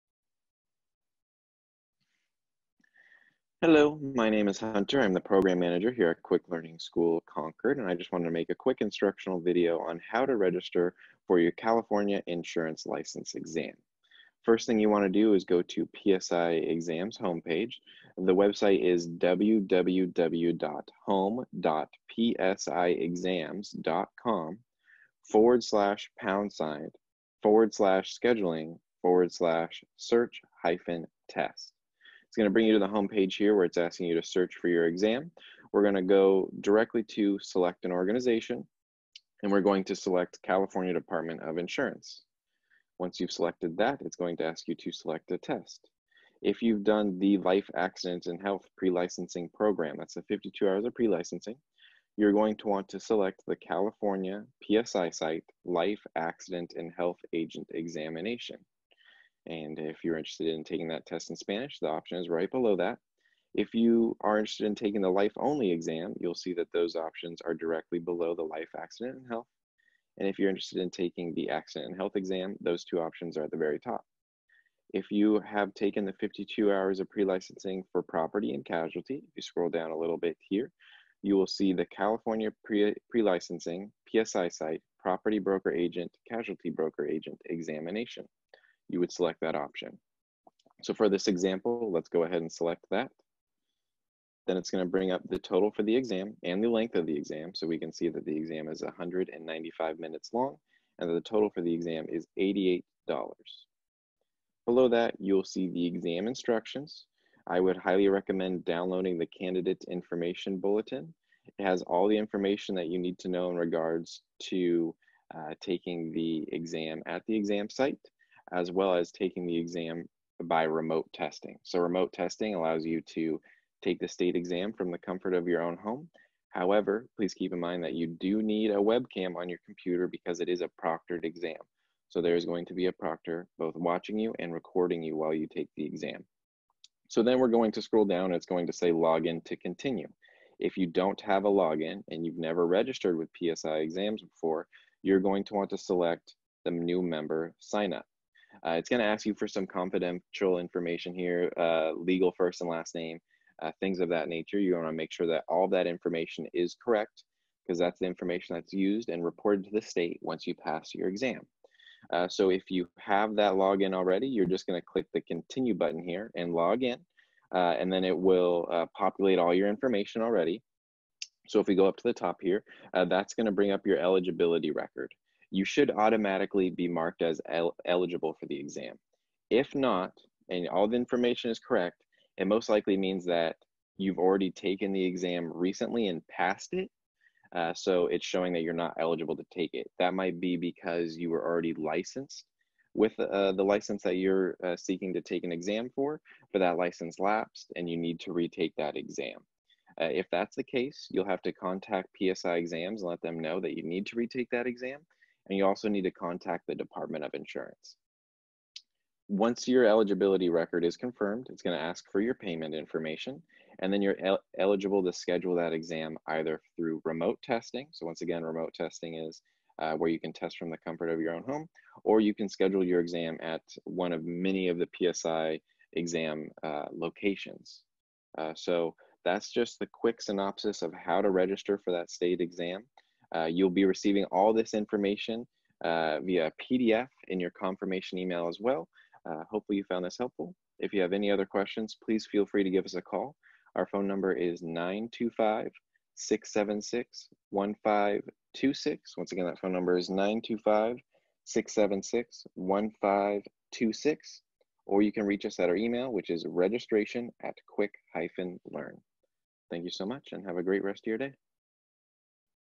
How to Schedule a California Insurance Licensing Exam - PSI Exams Instructions
Here is the link for the site to schedule your California insurance licensing exam:

We recommend open two windows - one for the video and the other for the site to schedule your exam.

As of August 23, 2020, the California Department of Insurance added another option to take the California Insurance licensing exam.  The options for testing include onsite PSI location testing or the new additional of remote testing in your own home.

There are changes that were made to the PSI site to accommodate for this alternative option.